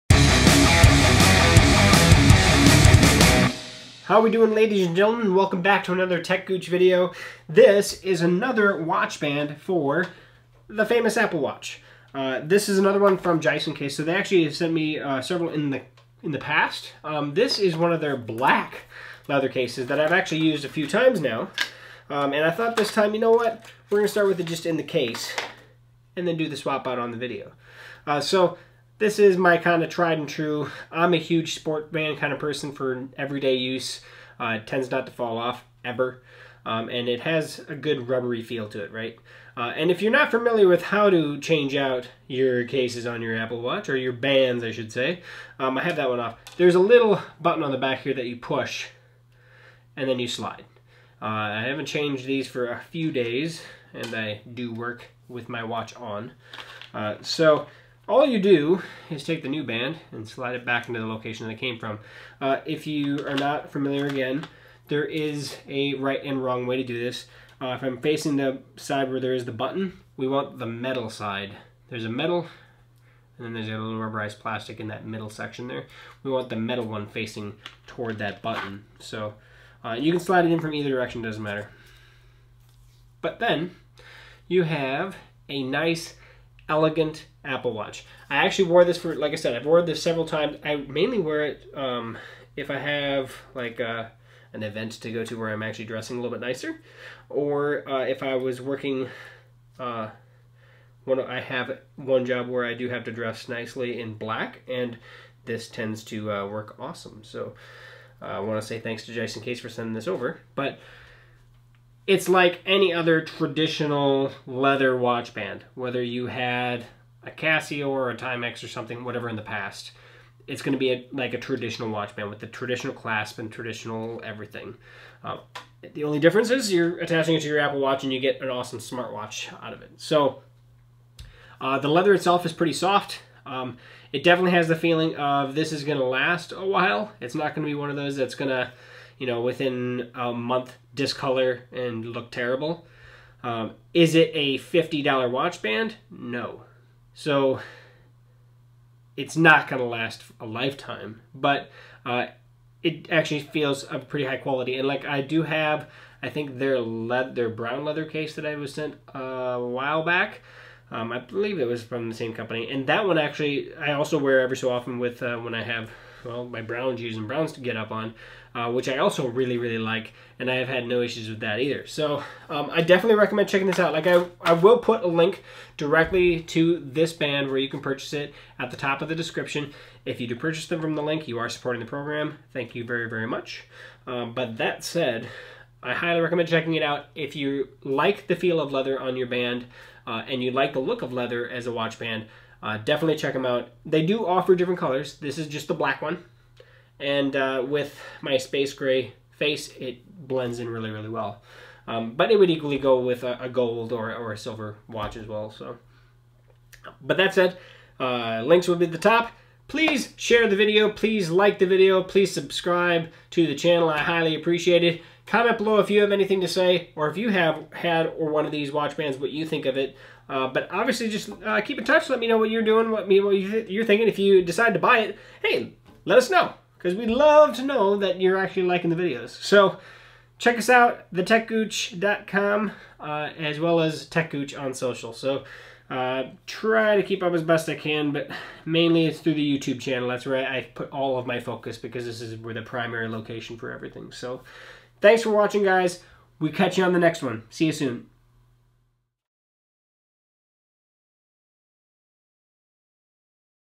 Jisoncase Apple Watch Leather Band Review
This Apple Watch band from Jisoncase is actually quite nice, and really accents the Apple Watch for that more professional look than the standard sport bands. Check it out at the link above.